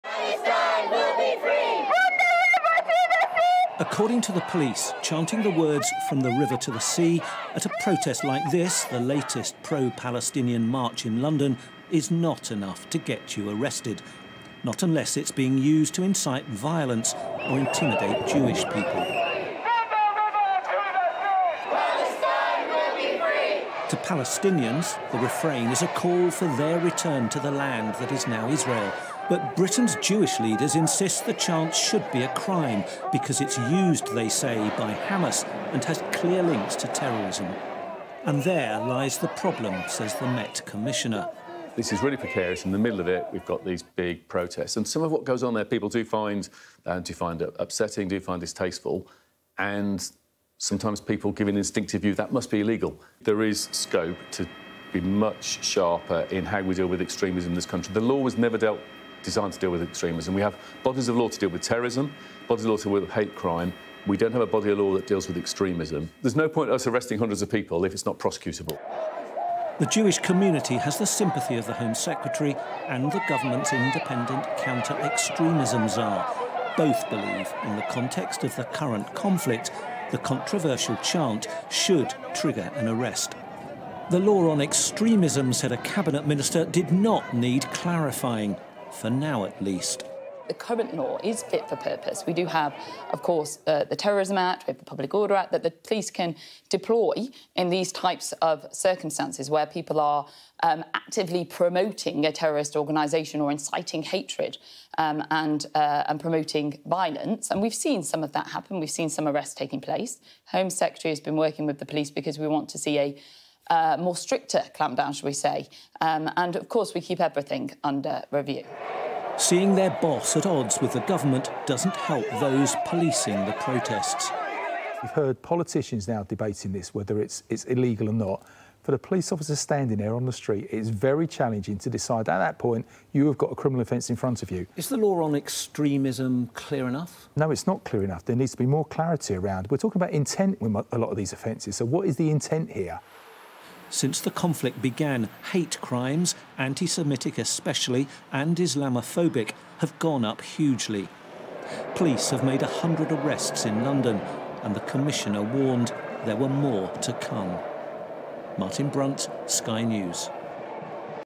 Israel-Hamas war: Met Commissioner calls for 'sharper' laws in dealing with extremism
Metropolitan Police Commissioner Sir Mark Rowley says his officers are working "ruthlessly" to arrest anyone who "steps over the line" by committing a hate crime - as tens of thousands pro-Palestine protesters took to the streets of London on Saturday.

Though the protest was largely peaceful, nine people were arrested and five people have been charged, but the Met have faced criticism of their policing of the march.